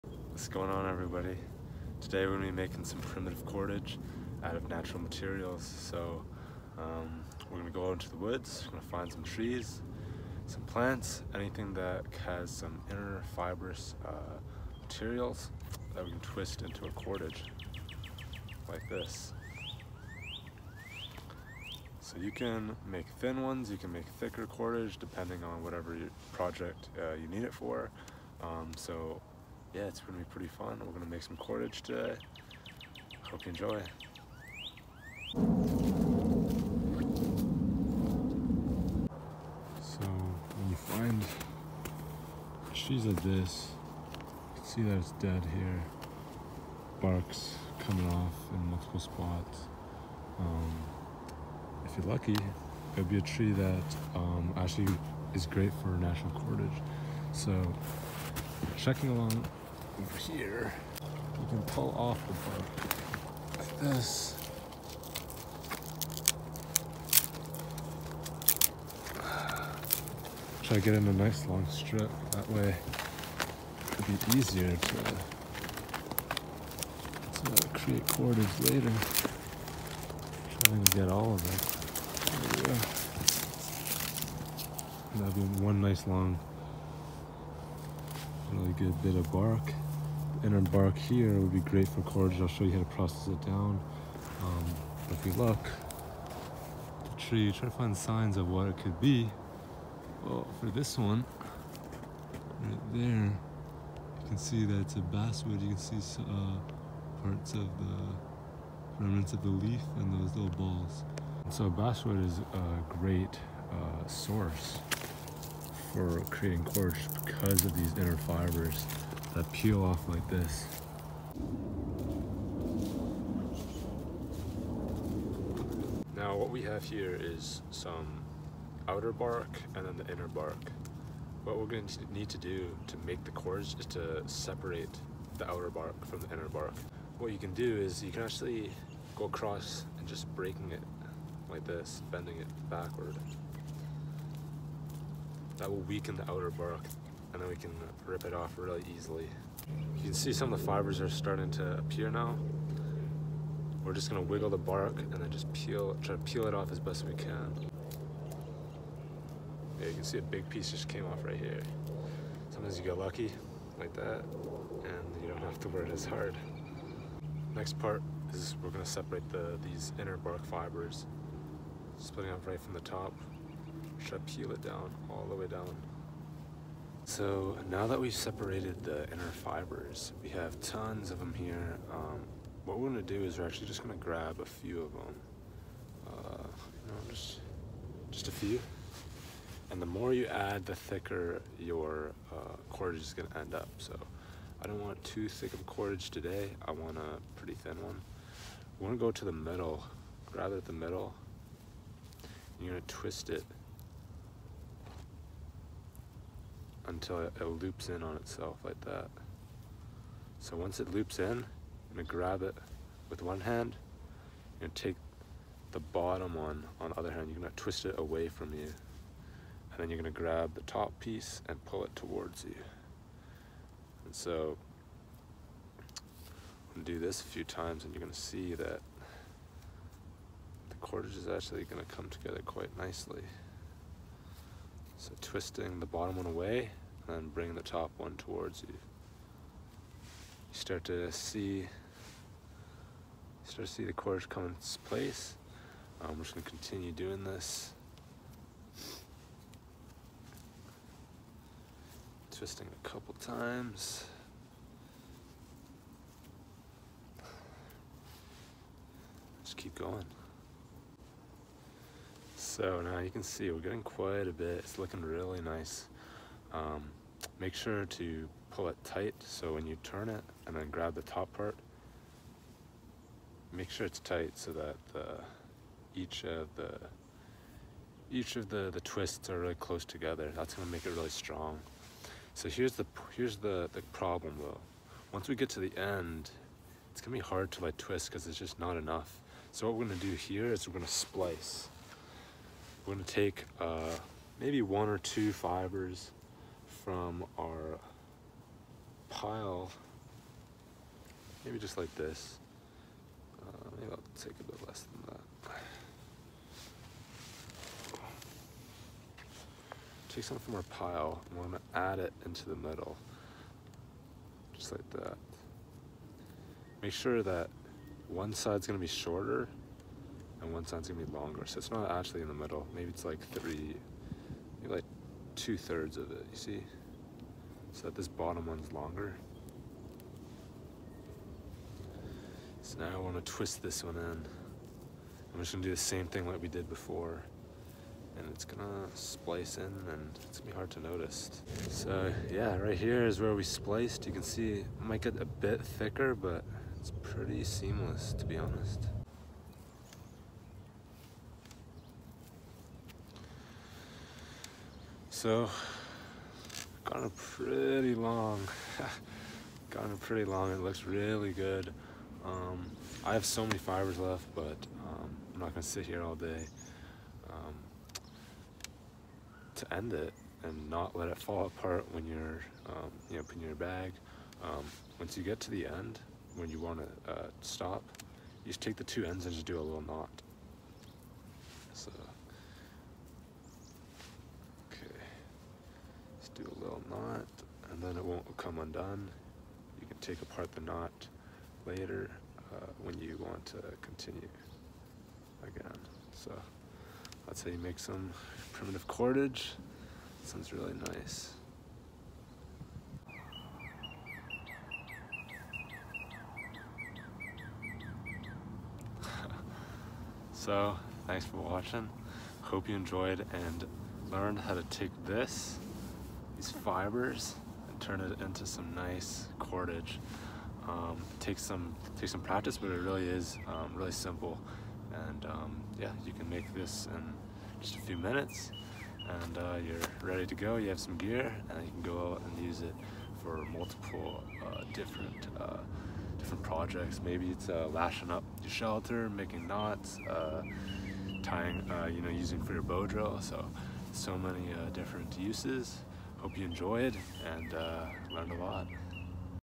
How I Make Primitive Cordage from Natural Materials (Using Basswood)
Join me on an outing to utilize the resources that nature provides. Today, an American Basswood (Tilia americana) provides its inner bark fibers to allow us to create cordage using the two strand twist technique. Try this out for yourself, and if you need any help, we can talk in the comments!